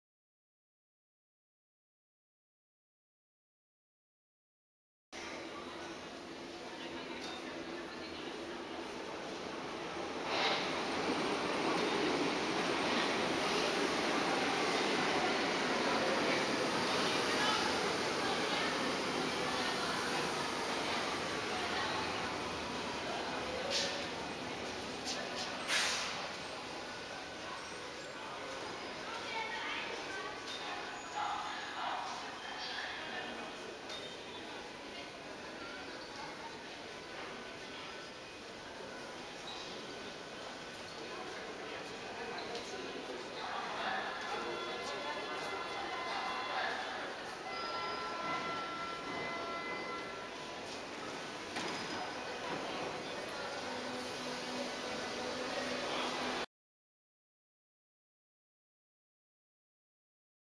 Berlin U-Bahn: U8 Alexanderplatz
Ein H-Zug der U8 Richtung Hermannstraße fährt in den Bahnhof Alexanderplatz ein.